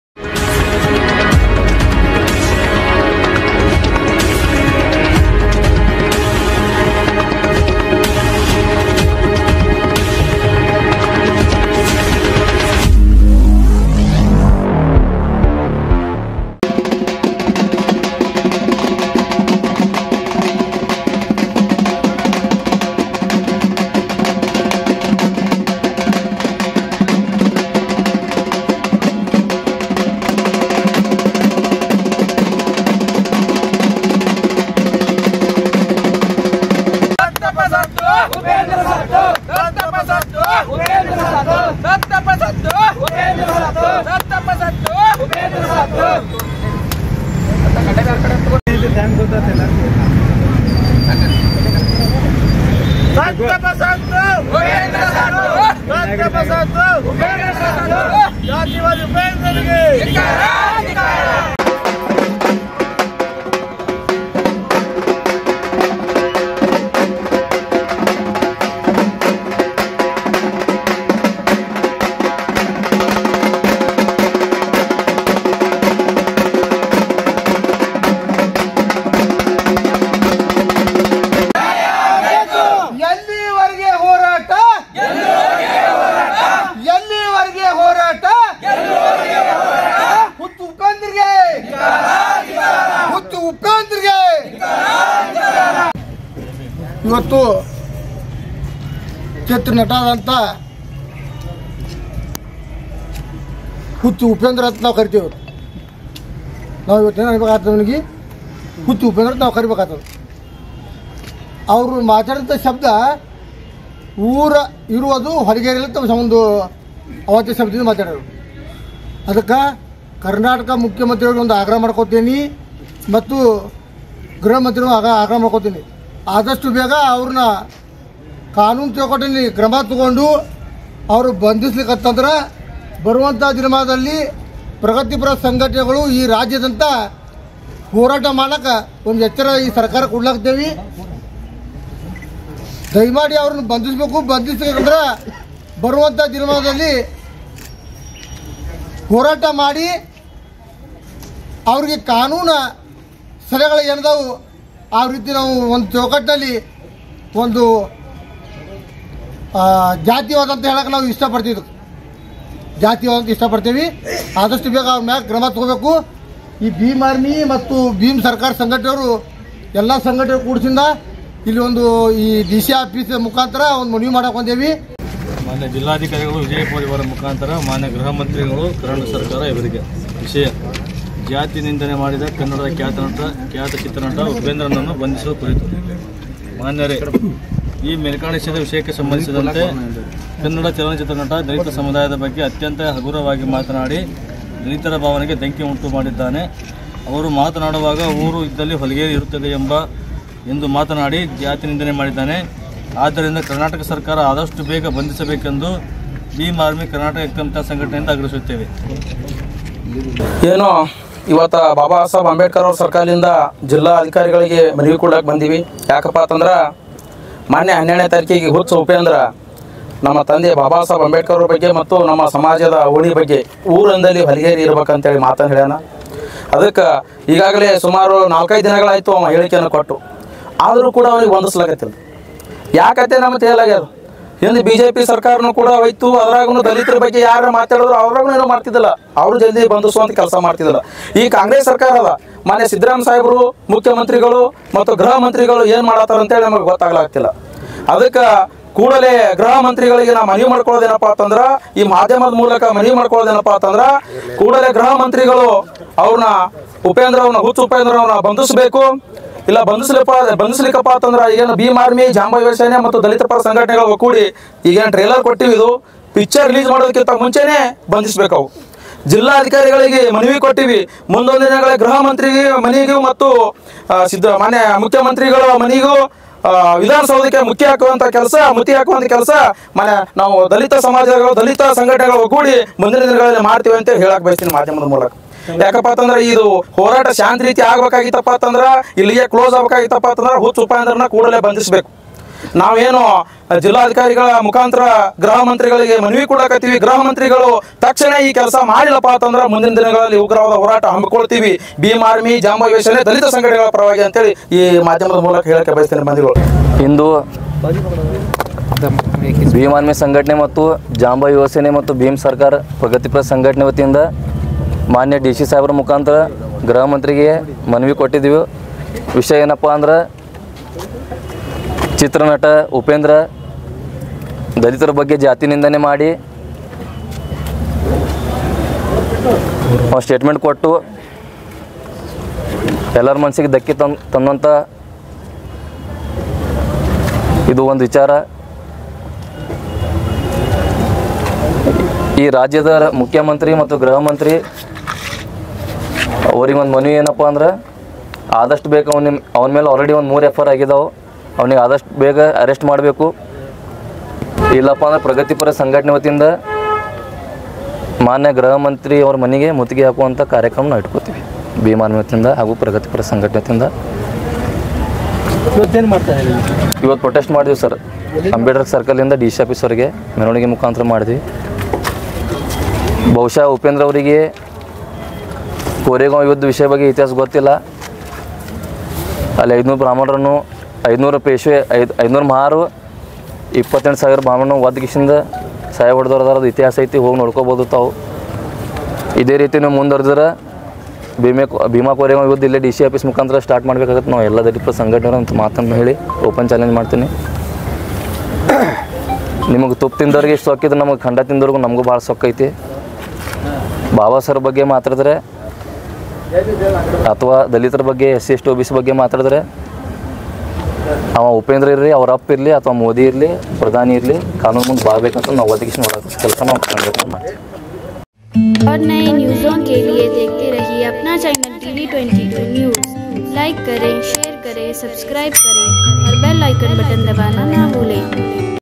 Kannda Film Actor Upendra Ke Khilaf Bhim Army Ka Aakrosh........!
Tv 22 News Delivers Reliable Information Across